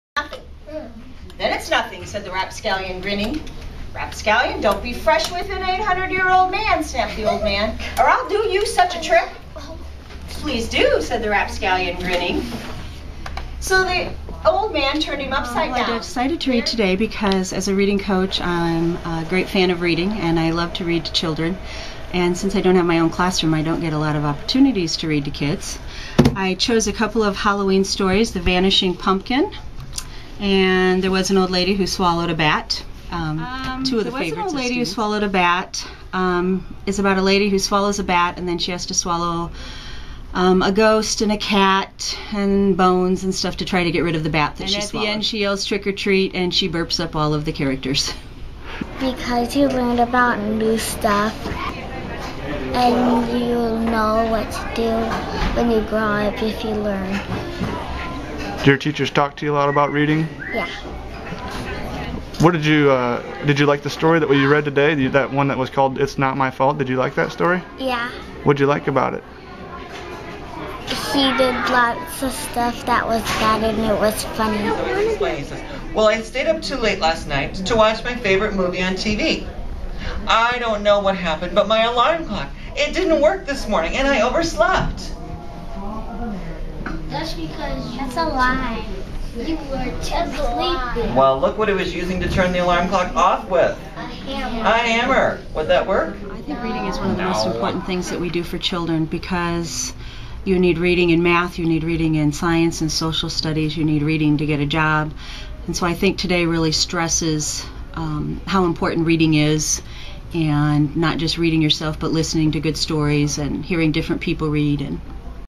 "America Reads: Reading is Contagious"
At Grant Wood Elementary School in Cedar Rapids Iowa, about 50 readers shared stories with the children during the all-day "America Reads: Reading is Contagious," event. They included teachers, former teachers and employees from the school's partner businesses.